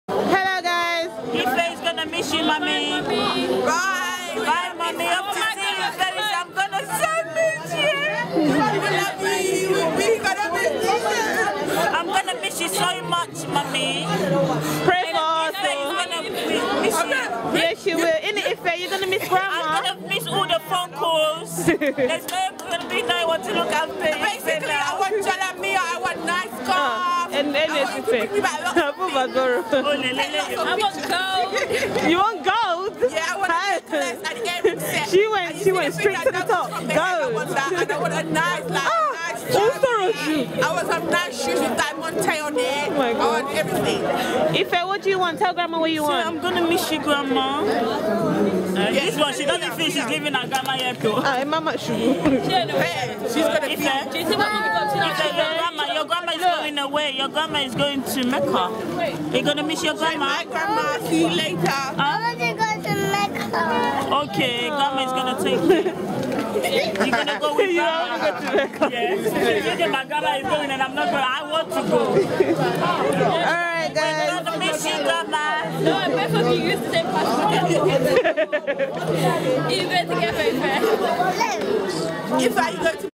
African Islamic Ceremony 16
African Islamic Ceremony - featuring special prayers for prospective Macca Pilgrims - UK November 2010.